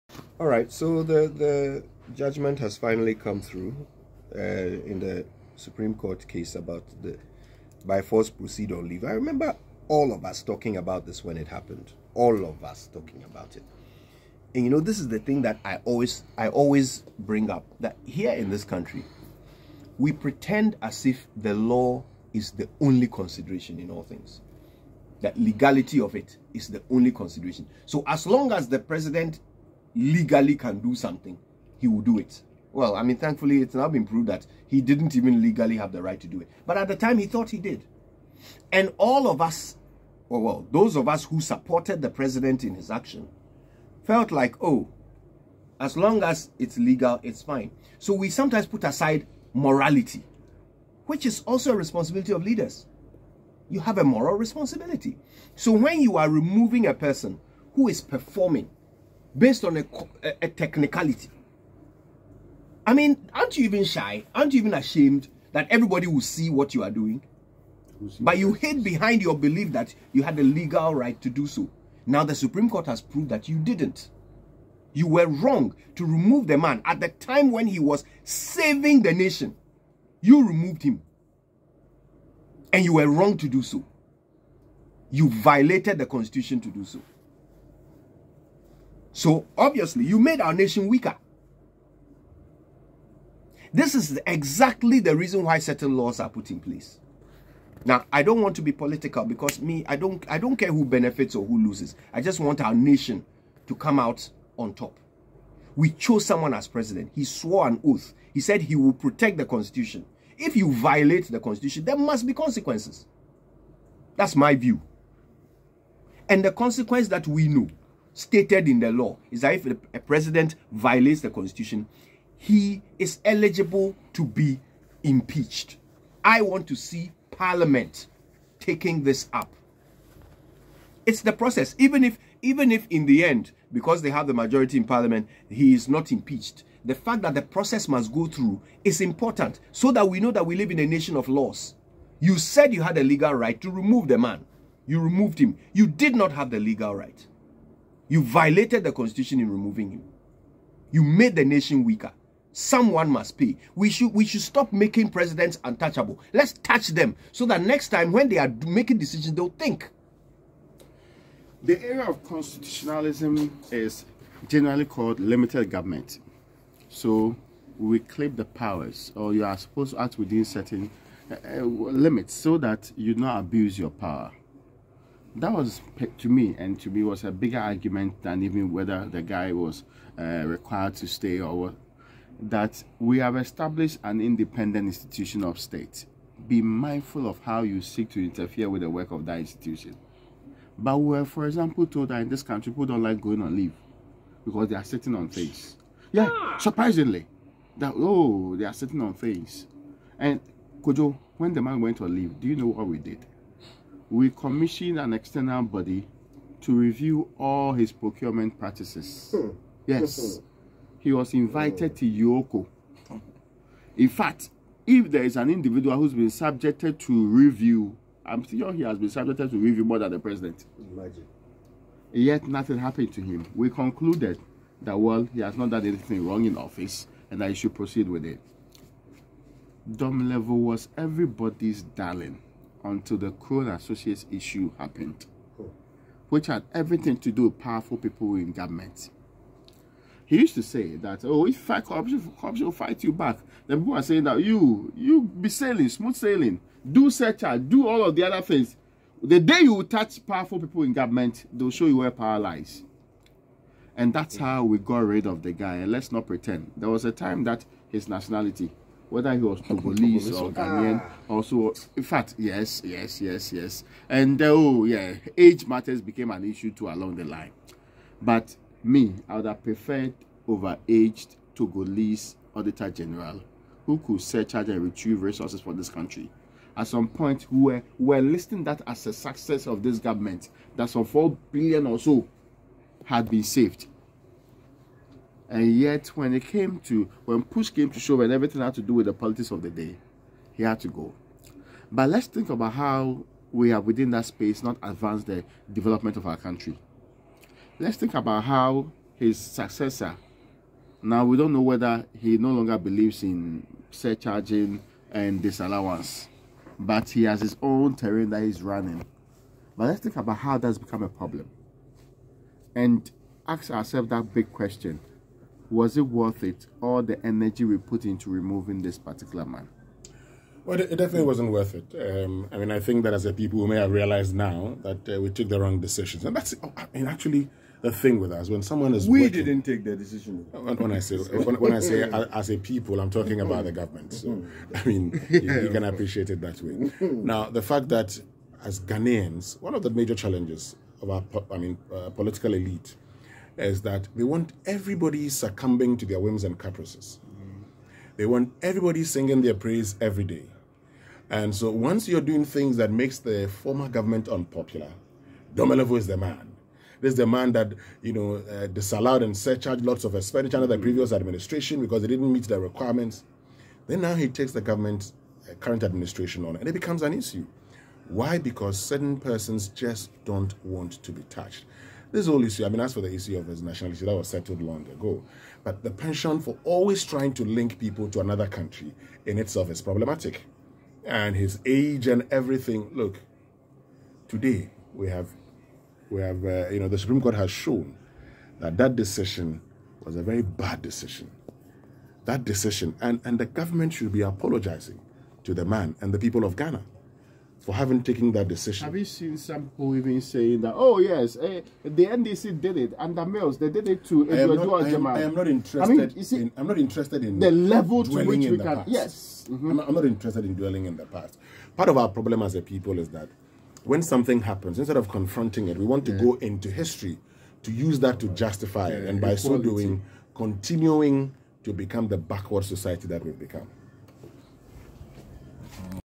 President was engaged in Domelevo's unconstitutional leave - is it right to impeach him?
Domelevo's unconstitutional leave; the President was engaged in something unconstitutional - is the time right to impeach him?

- Kojo, Raymond, and Winston discuss Domelevo's leave and matters arising.